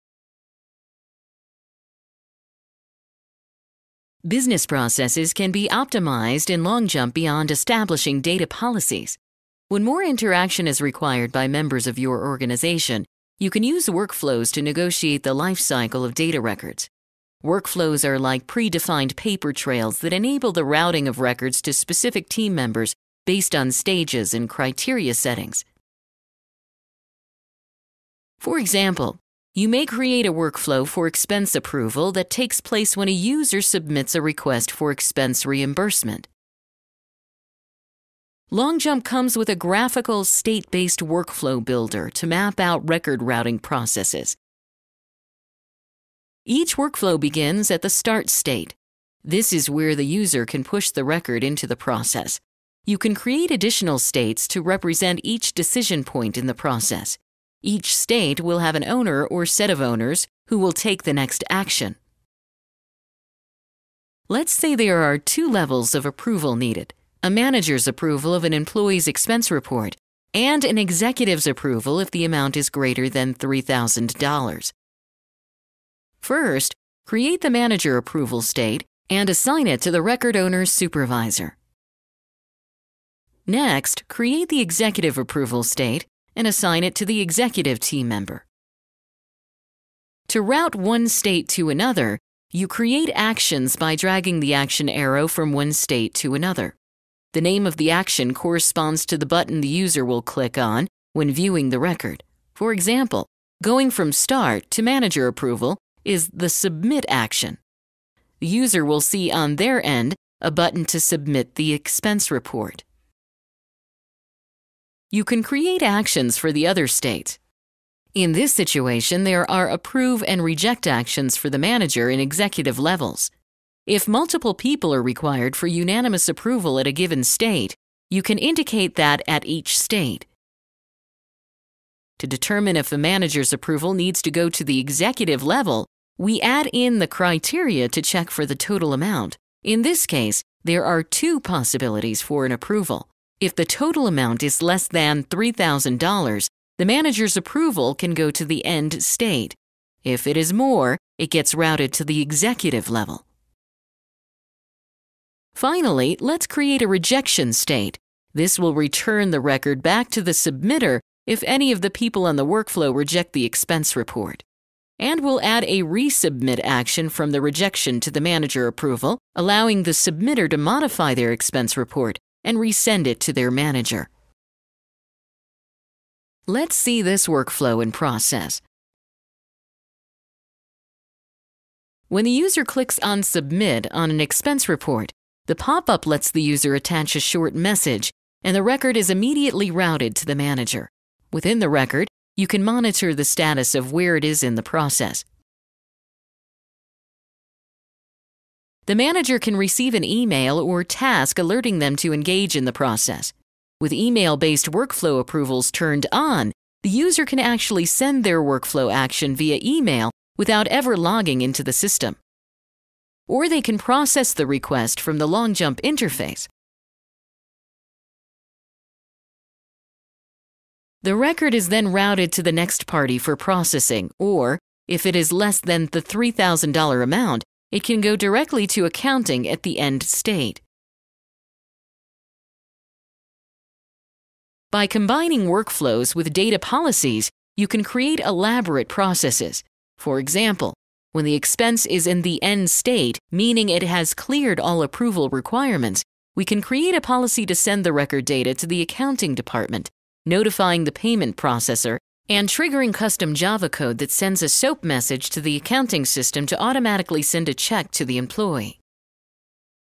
Workflows - LongJump PaaS
LongJump comes with a graphical, state-based workflow builder to map out record routing processes to design more interaction for members of your organization. Workflows help to negotiate the lifecycle of data records and act like pre-defined paper-trails that enable the routing of records to specific team members based on stages and criteria settings.